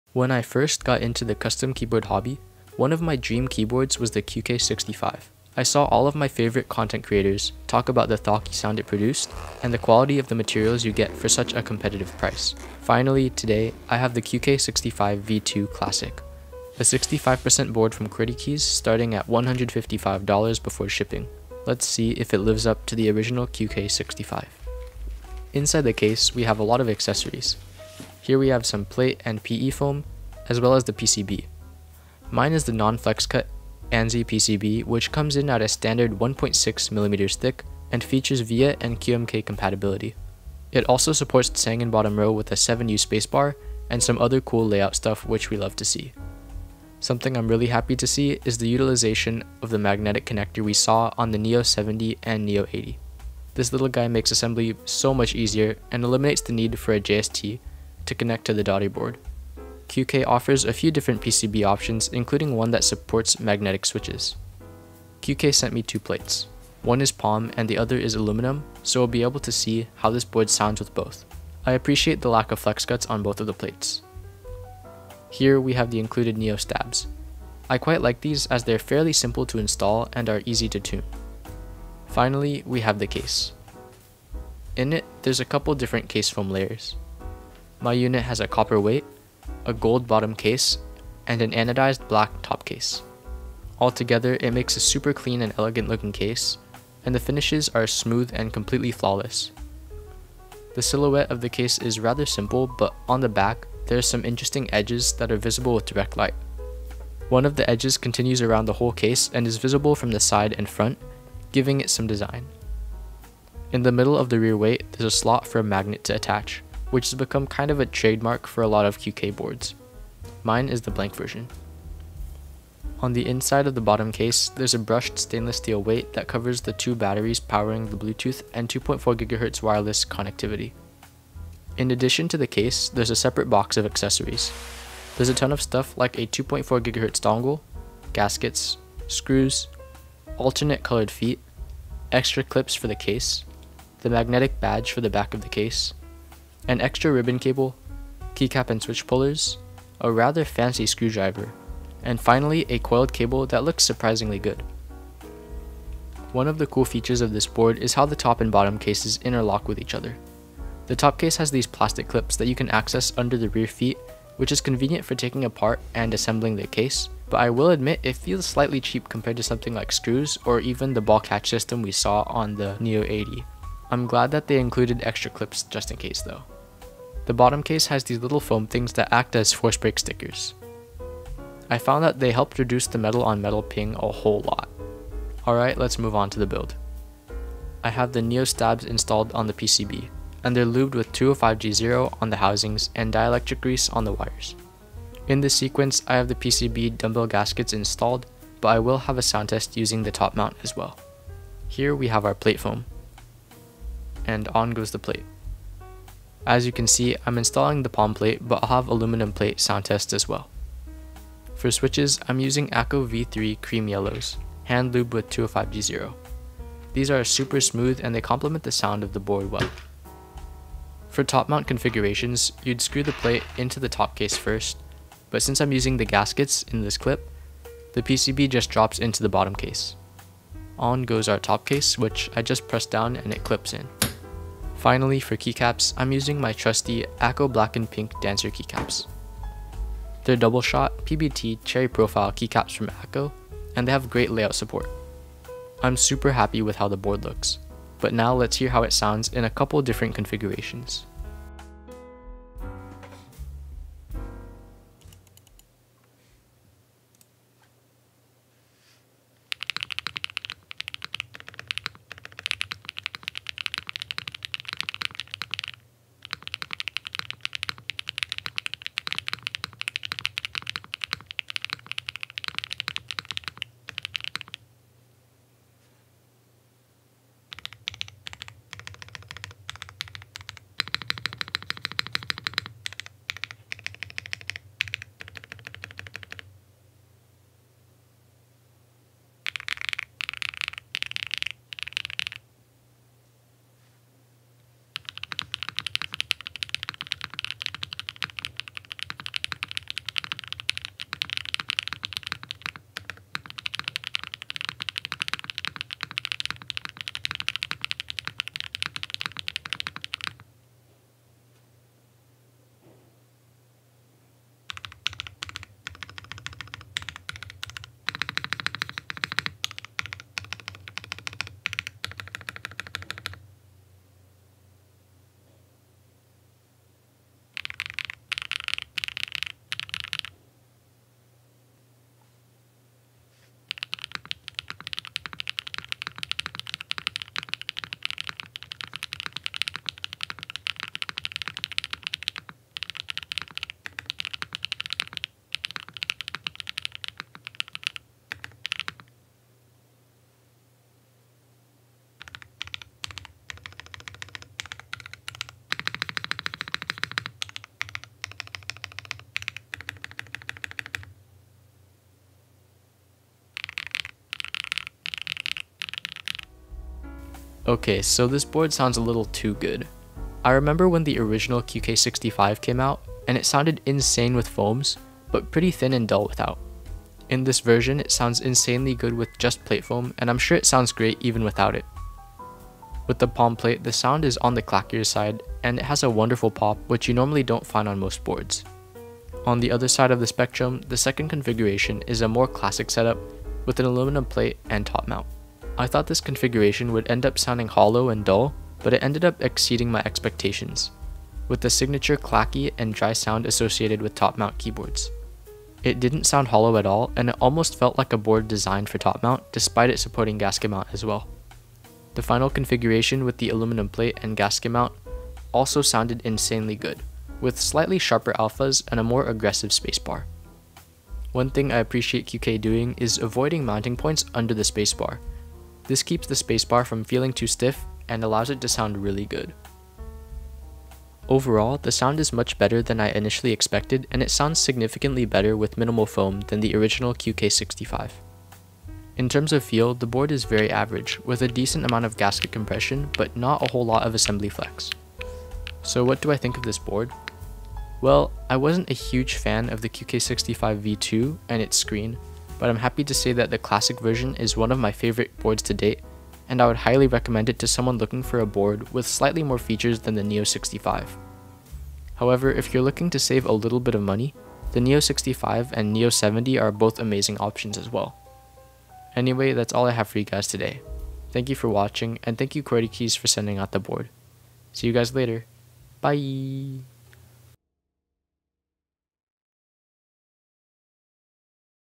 This Custom Keyboard is INSANE | QK65v2 Classic Build and Sound Tests
Specs:
QK65v2 Classic
Pom/Alu Plate
Akko Cream Yellow (205g0)
Akko Black and Pink the Dancer Keycaps
Plate Foam